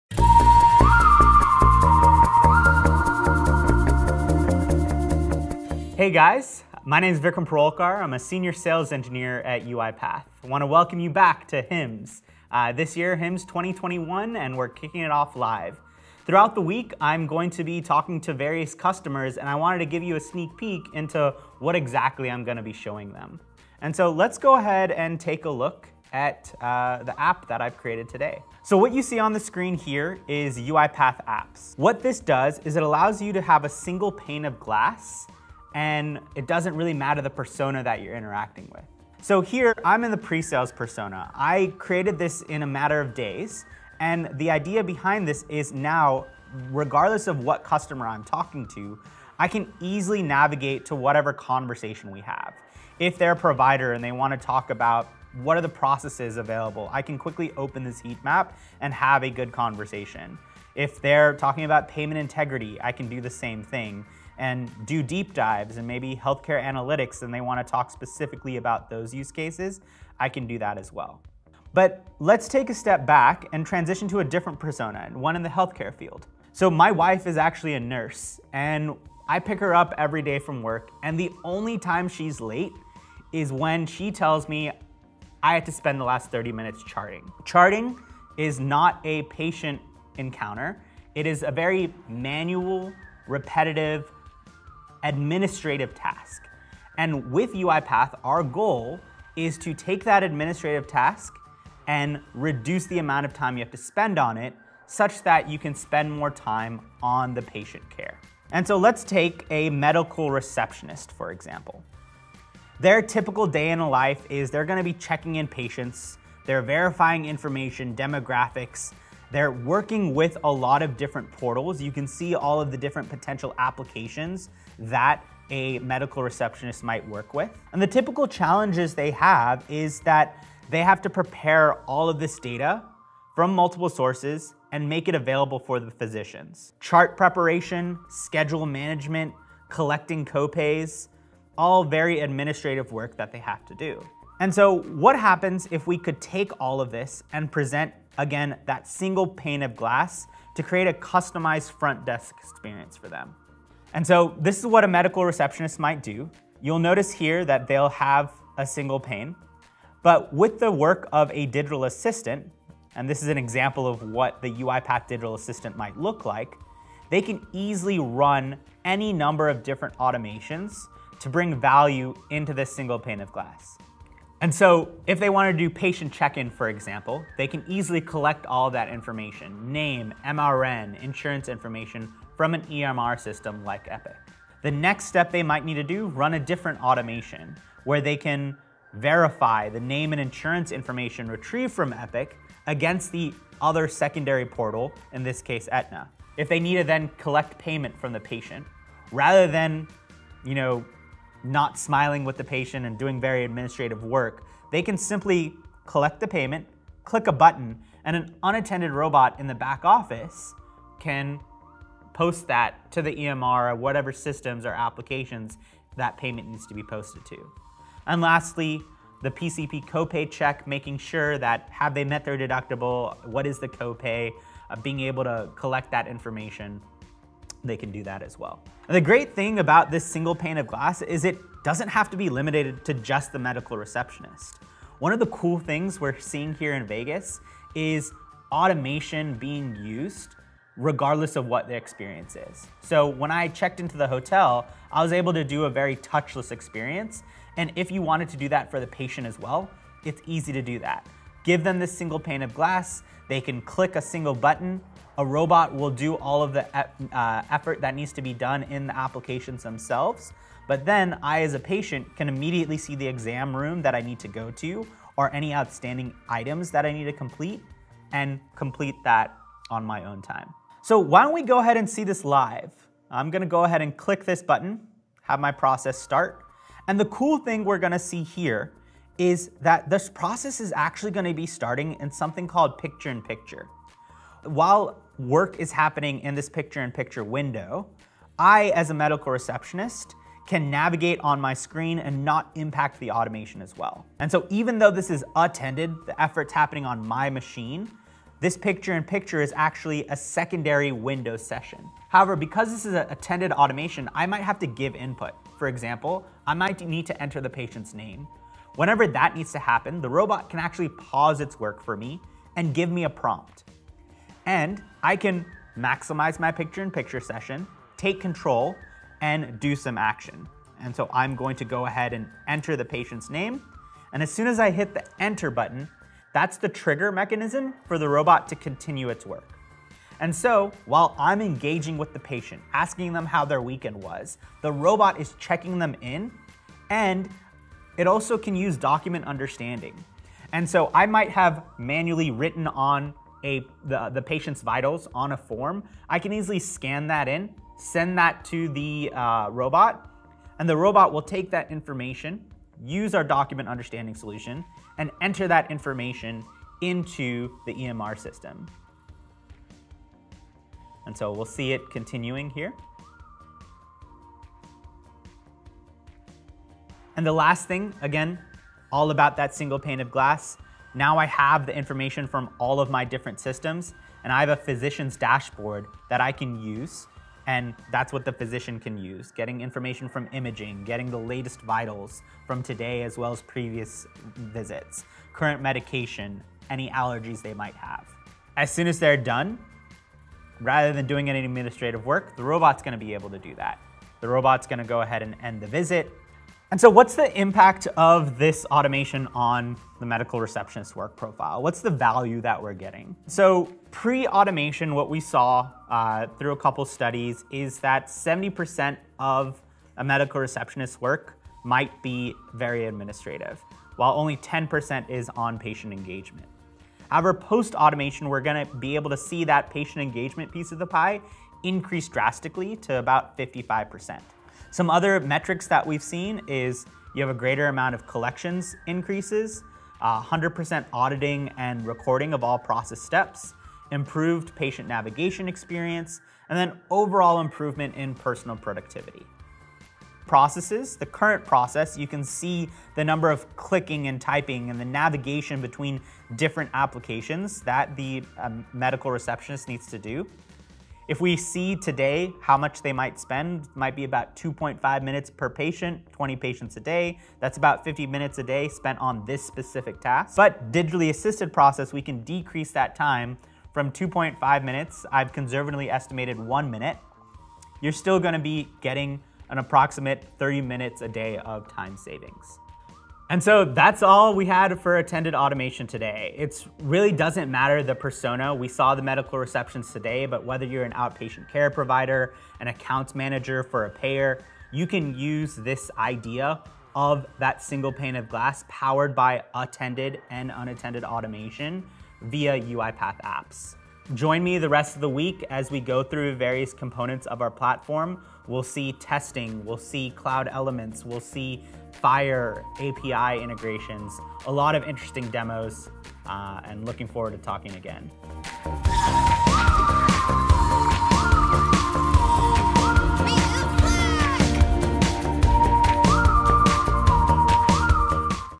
Attended automation and UiPath Apps in Healthcare
Join us for the first dedicated UiPath Healthcare Summit at FORWARD IV, Oct 4th

Excited to be back to talk about how the UiPath Platform can be used in the Healthcare industry. Today's session is all about UiPath Apps and Attended Automation (Single Pane of Glass).